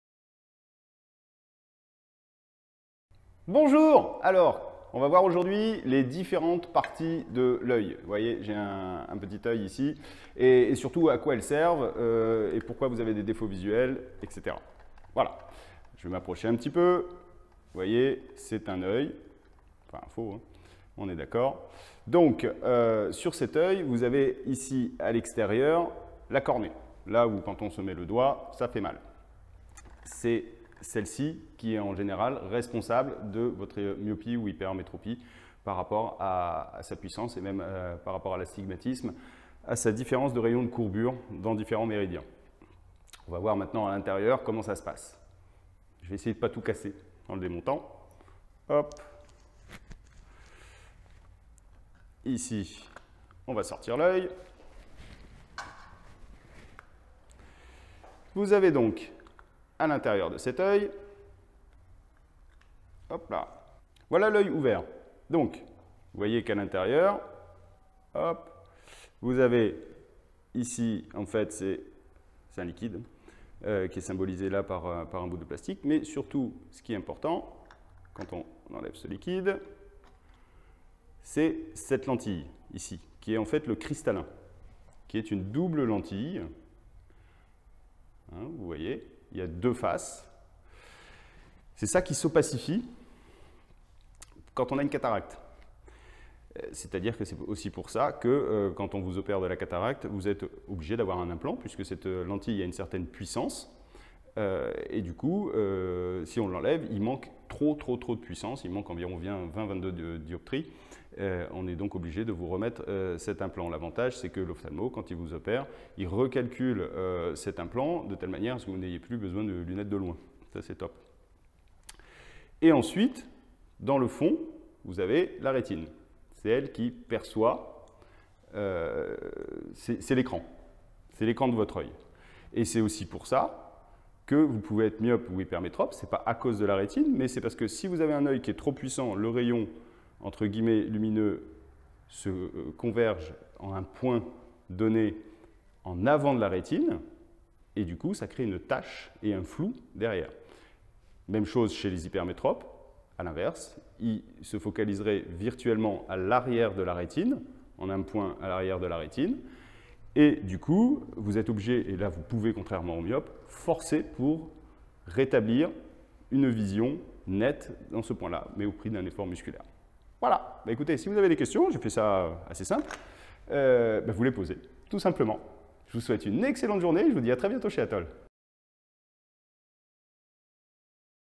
Les différentes parties de l'œil par Atol mon opticien Istres
Grâce à une reproduction d'œil découvrez les différentes parties de orte oeil et à quoi elles servent.